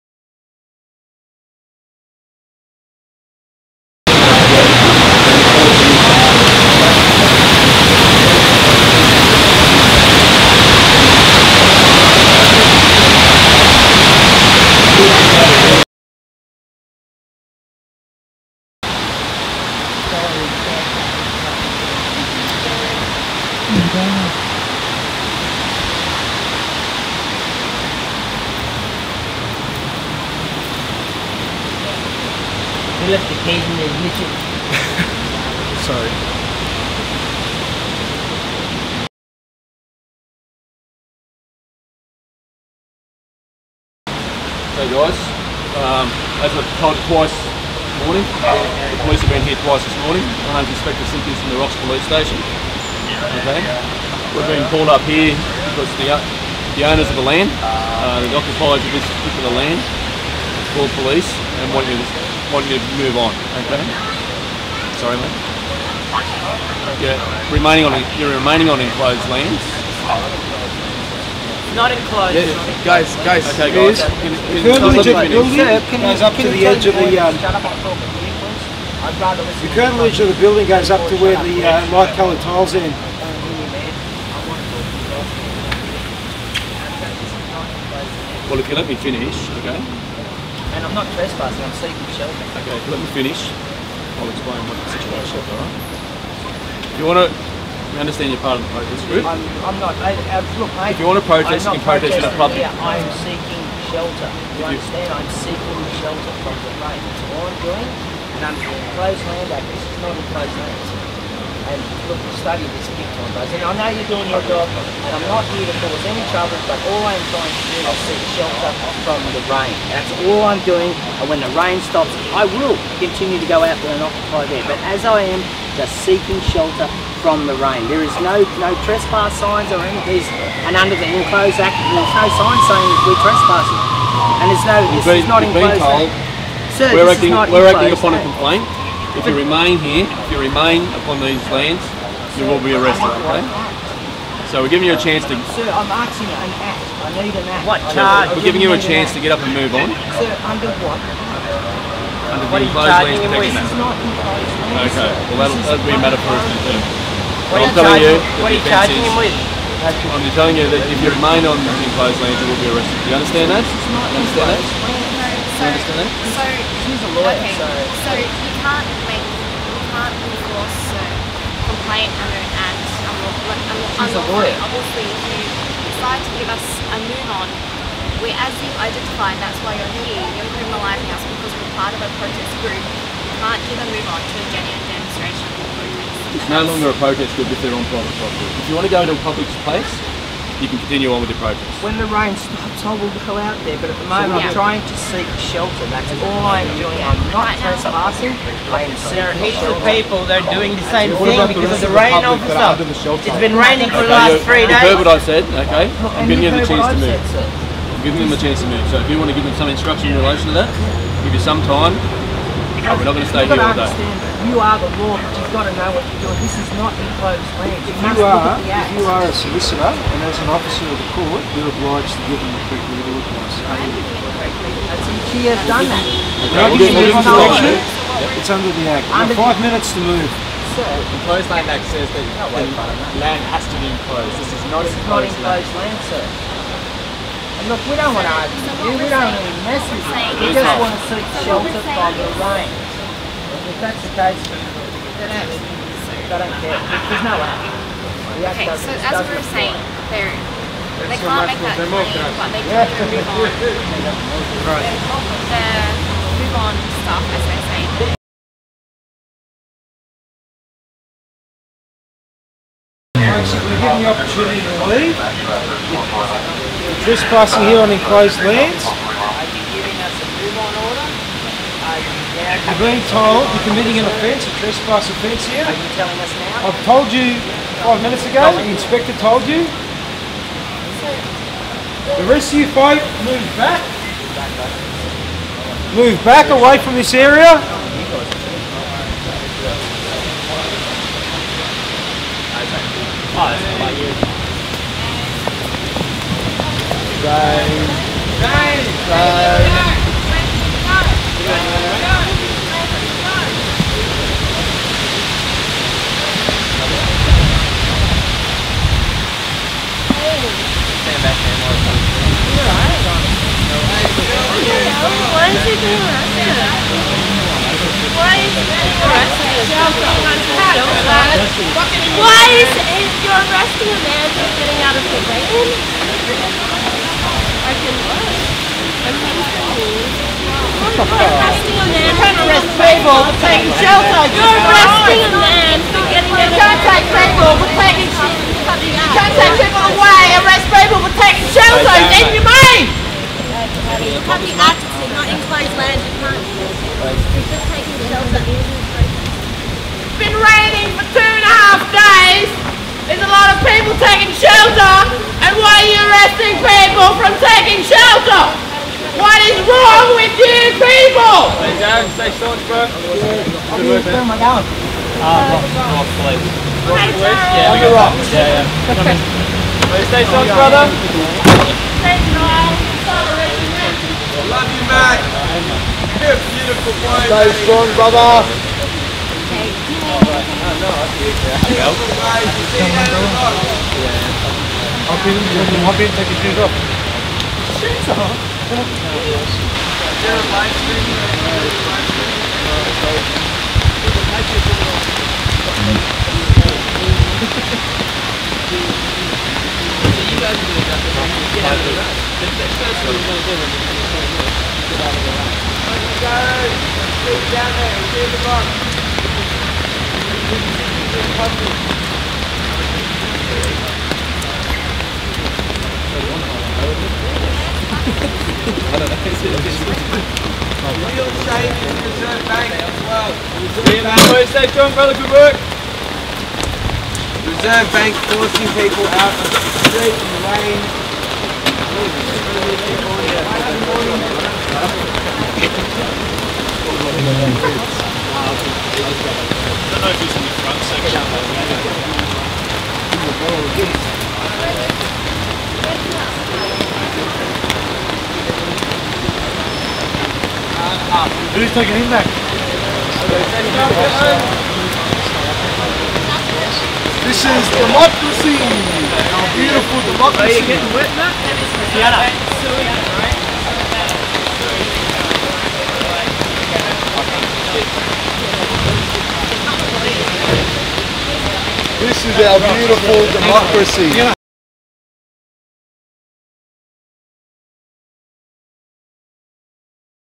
Occupy Sydney Arrests 23rd November 2011
Three people were arrested at Occupy Sydney today because they did not comply with a "Move On" order despite that fact they were sheltering from the heavy rain in non-enclosed lands.